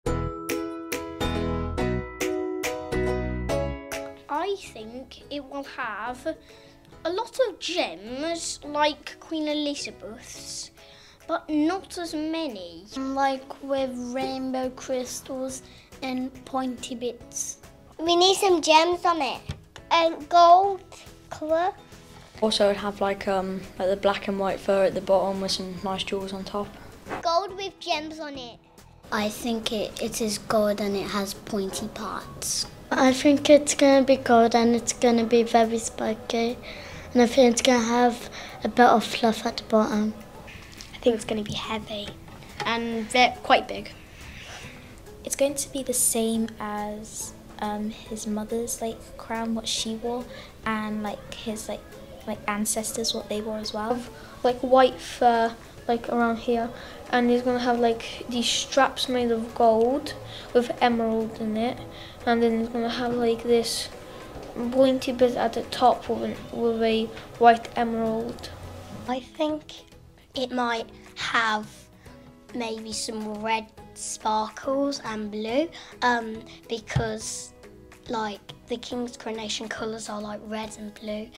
School children tell us what they think the King's Crown will look like
With thanks to Hollymount School in Worcester, The Orchards School in Bromsgrove and The Black Pear Trust.